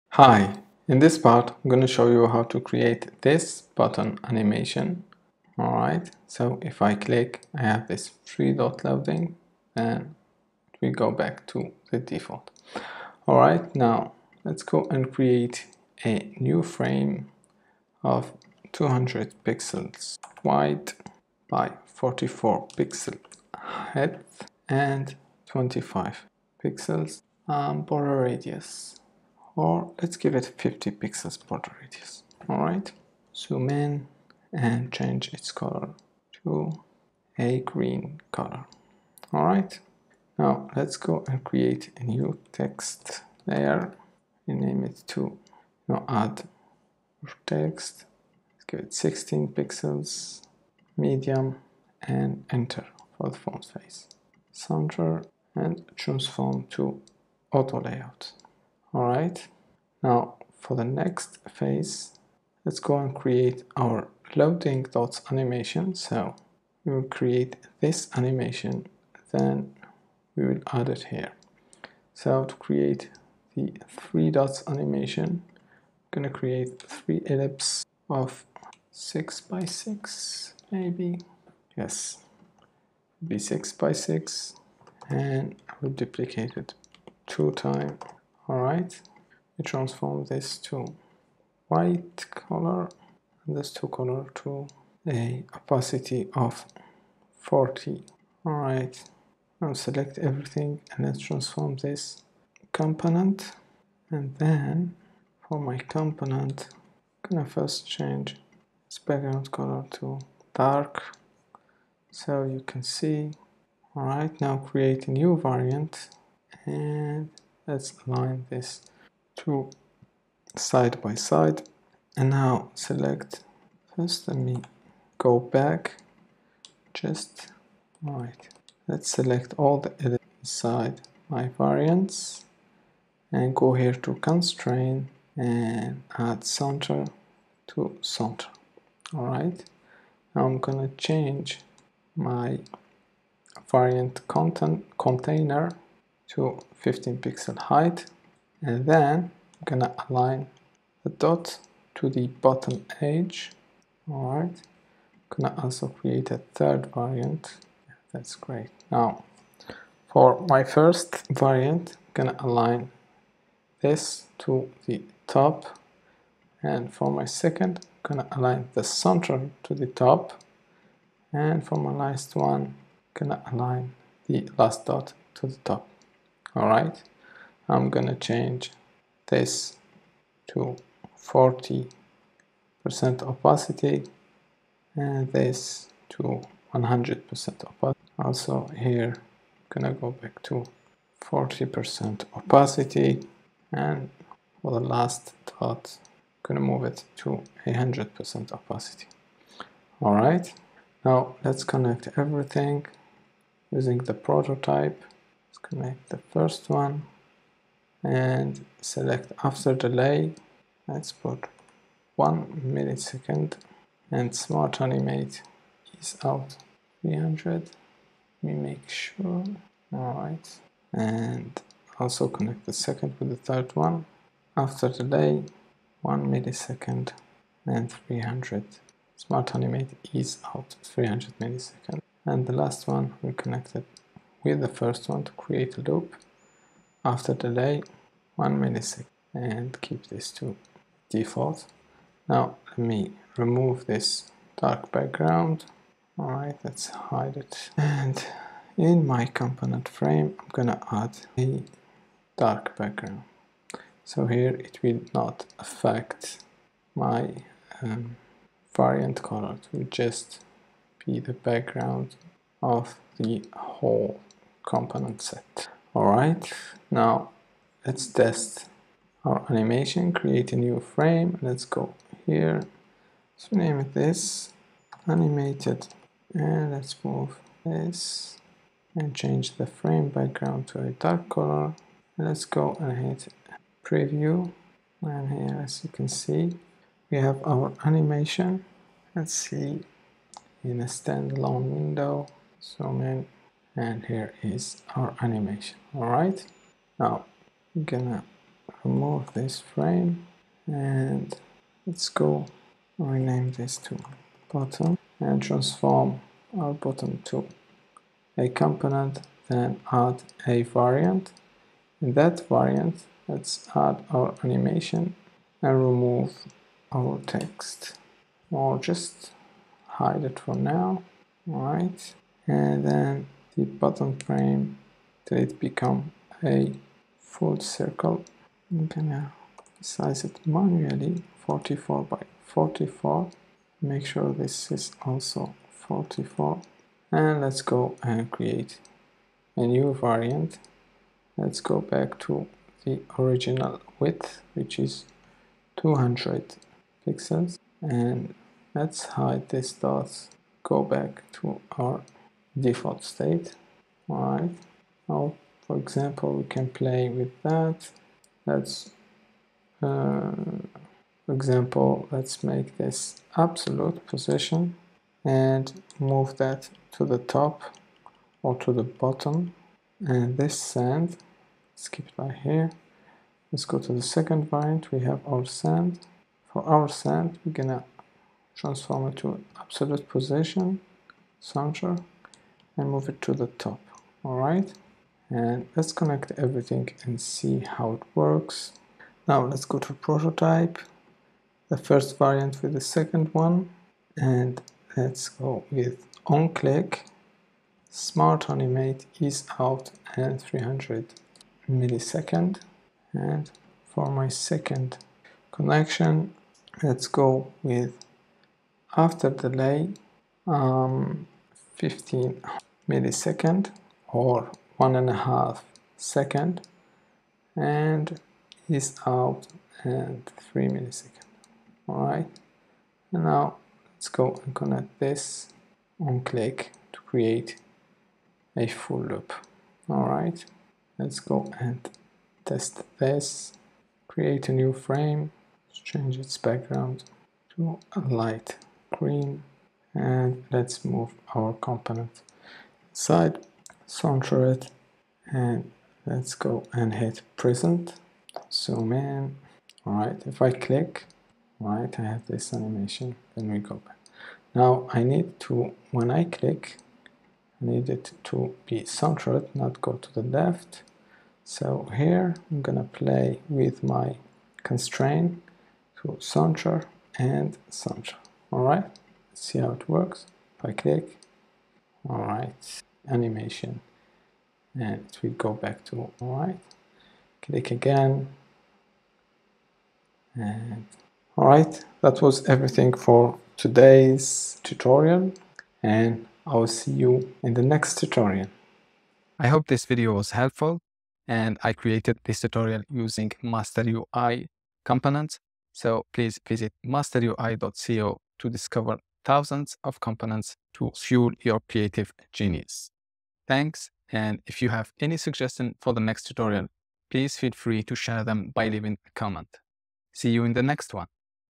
Create a Creative Figma Button loading Animation - Loading Animation Tutorial
📥 **Resources mentioned this tutorial** 📥

🚀 Unlock your design creativity with our tutorial on "Create a Creative Figma Button Loading Animation - Loading Animation Tutorial"! Dive into the intricacies of crafting visually stunning loading animations for your buttons in Figma. Whether you're a design enthusiast or a seasoned professional, this tutorial is your guide to adding that extra flair to your user interface. Join us on this journey to master the art of Figma loading button animations, bringing innovation to your designs. 💻✨